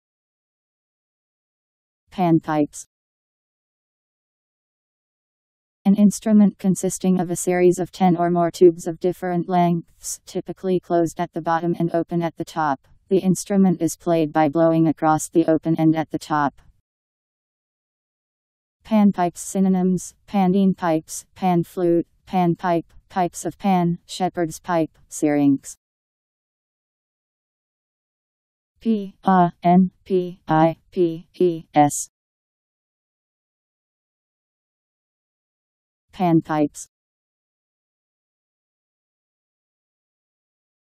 Panpipes Meaning
Video shows what panpipes means. An instrument consisting of a series of ten or more tubes of different lengths, typically closed at the bottom and open at the top. The instrument is played by blowing across the open end at the top.. panpipes synonyms: Pandean pipes, pan flute, pan pipe, pipes of Pan, shepherd's pipe, syrinx.  Panpipes Meaning. How to pronounce, definition audio dictionary. How to say panpipes. Powered by MaryTTS, Wiktionary